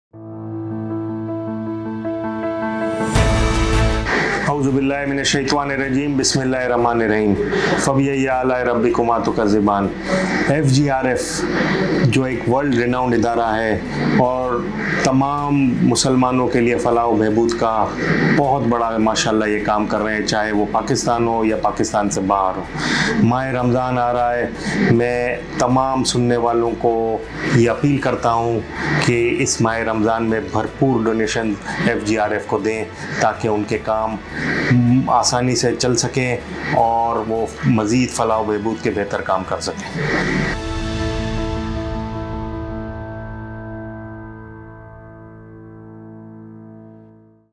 Mr. Irfan Wahid (CEO of GFS Group) I Promoting The Noble Cause of FGRF - Faizan Global Relief
The CEO of GFS Builders and Developers, Mr. Irfan Wahid, is a business tycoon and an active philanthropist. This time he is appealing to the viewers and audience to support FGRF - Faizan Global Relief, a charitable welfare foundation in Pakistan that currently are working on earthquake disaster affectees of the Northern side.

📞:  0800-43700